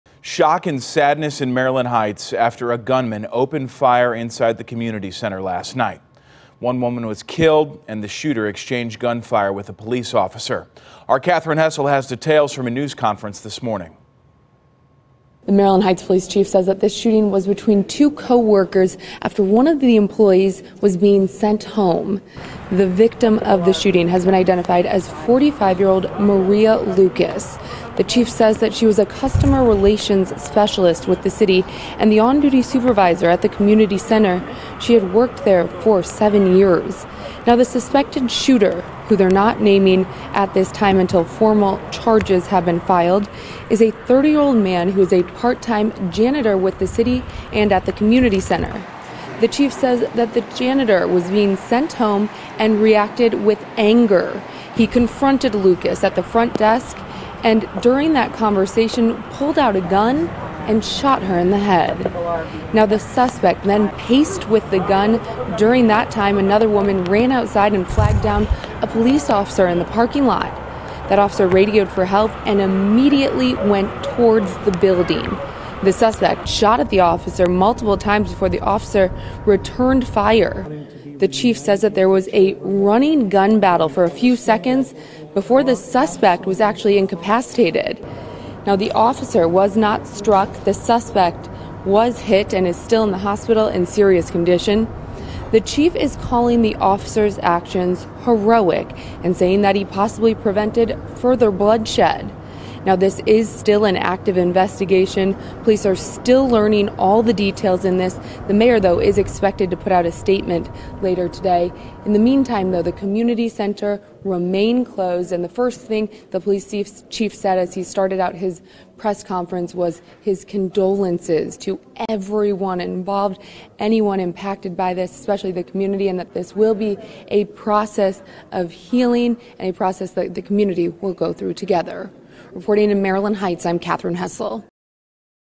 Shooting at Maryland Heights Community Center - One woman dead
Police confirm one woman is dead after a shooting at the Maryland Heights Community Center Monday night, with a suspect in custody after an exchange of gunfire with a Maryland Heights Police officer.